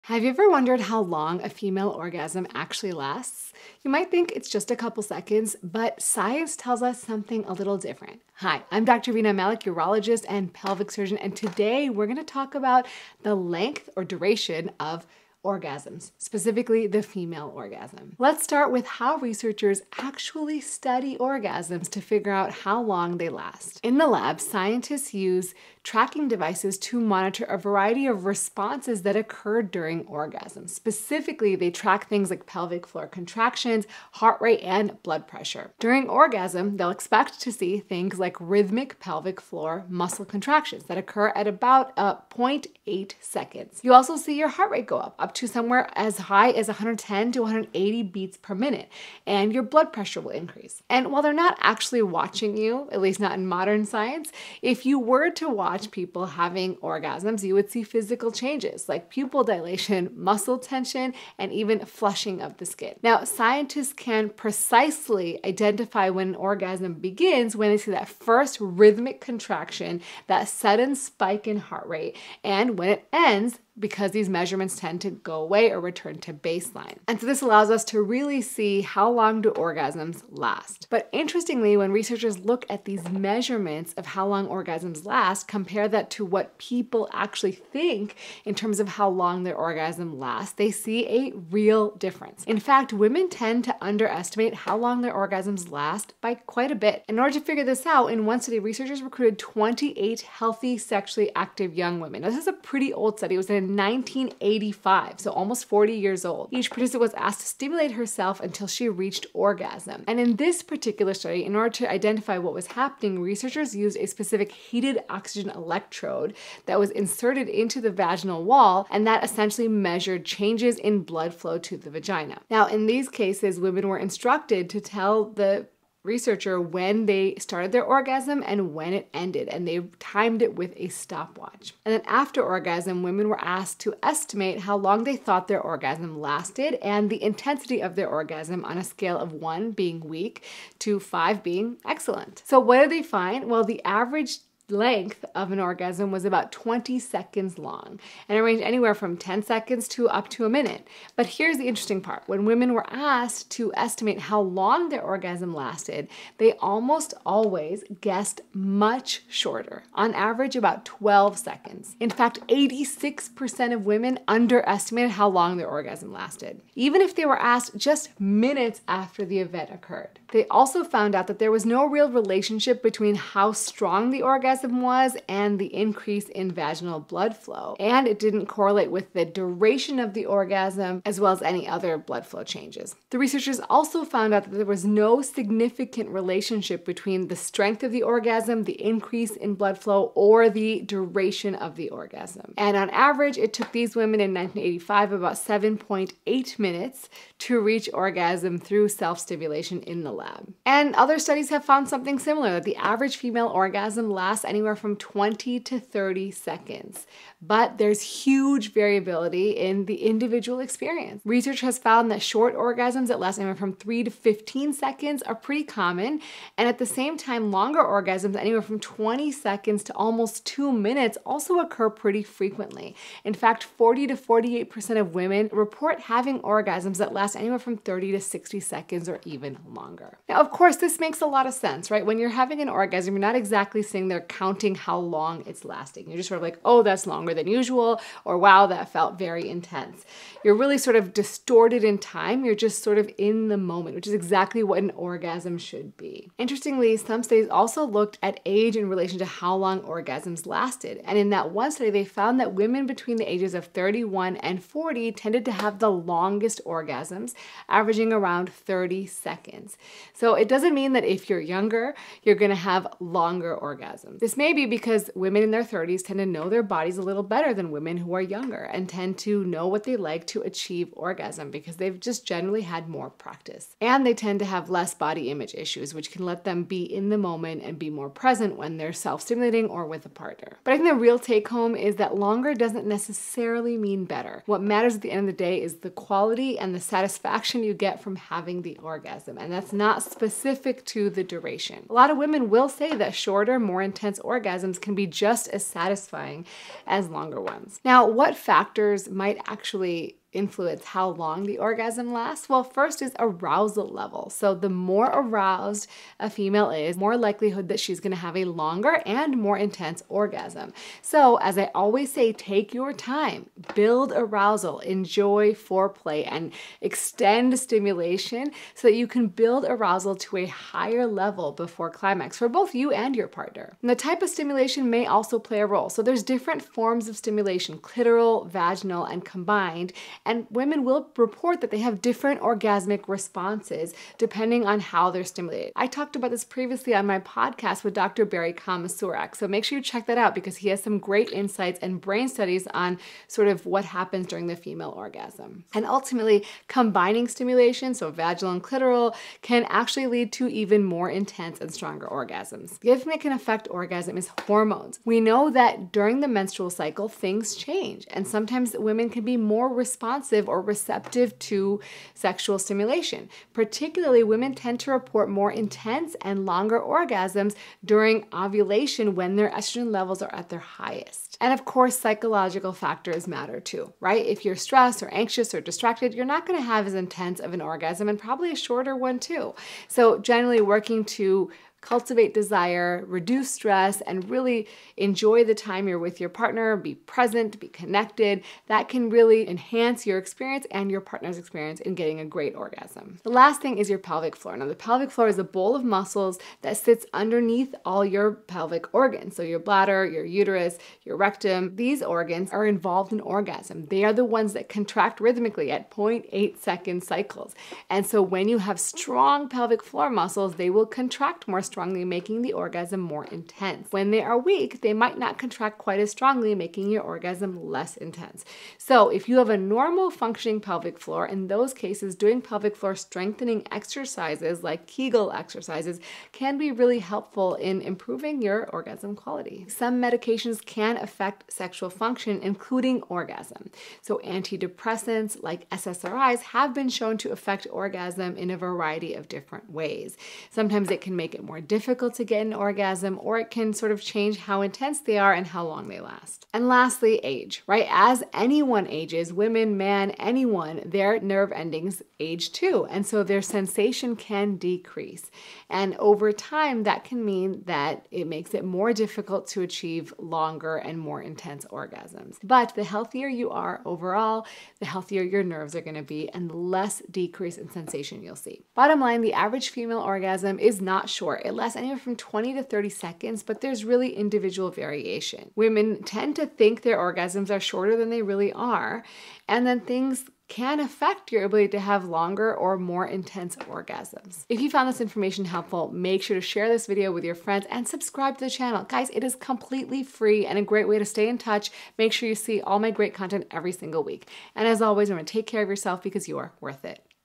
How Long Does a Female Orgasm Actually Last? The Answer Will Surprise You
Think you know how long a female orgasm lasts? You might be surprised. In this video, Dr. Rena Malik explains what science reveals about the duration of female orgasms, how researchers measure them, and the factors that can make them longer or shorter. Watch now to learn the truth about pleasure, backed by research.

Download my top 10 guide to maximizing pleasure - Better Sex,Better Life

Orgasm in women in the laboratory—quantitative studies on duration, intensity, latency, and vaginal blood flow.

▶️Chapters:
00:00 Introduction
00:24 Study about orgasms
02:29 Average orgasm length
03:19 Orgasm though self-stimulation
04:53 Factors that affect orgasm
05:03 Arousal
05:56 Hormones
06:34 Pelvic Floor
07:11 Medications
07:29 Age
07:50 Bottomline